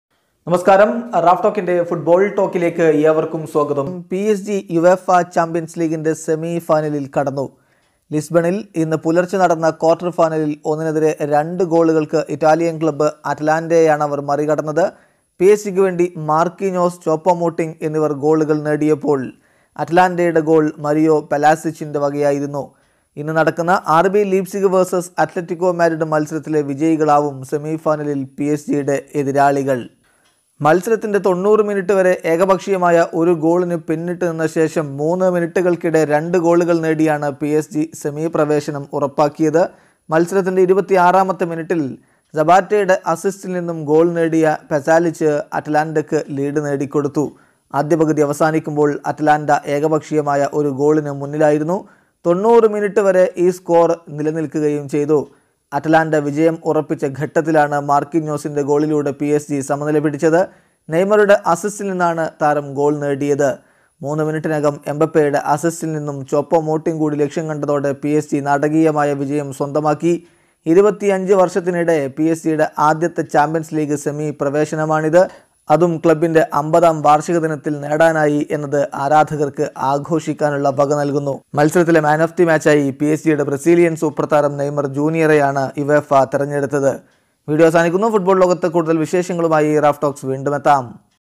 നാടകീയം, അവിശ്വസനീയം : PSG സെമിയിൽ | PSG vs Atalanta
Psg vs Atalanta
UEFA champions league
12/8/20 ന് ടക്കുന്ന മത്സരത്തിൻ്റെ റിപ്പോർട്ട്